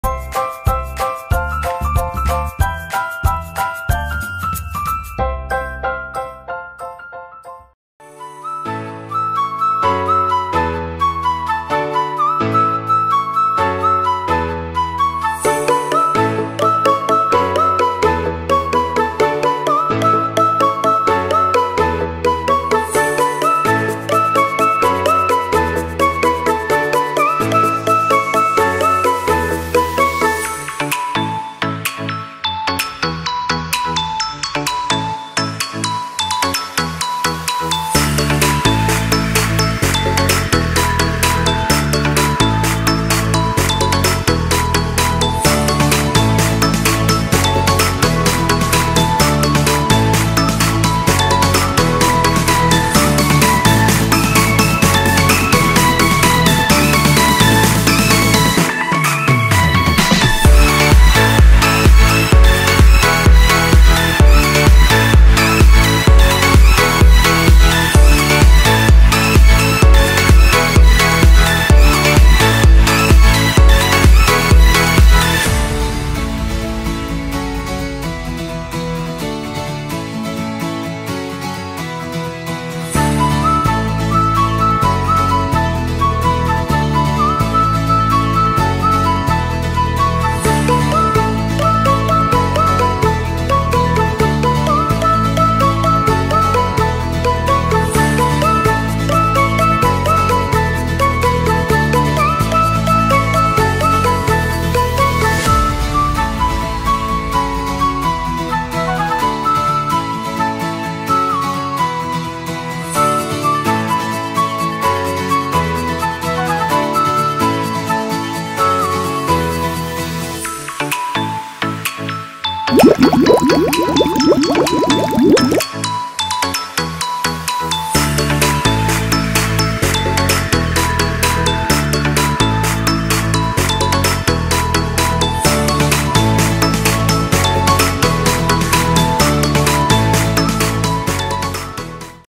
Pool Party Time! | KloKick Inflatable Pool
Thank you KloKick for sending us this awesome inflatable pool! We absolutely loved it!

Hello everyone!
Also turn on our notifications 🛎so you know when we upload a new video!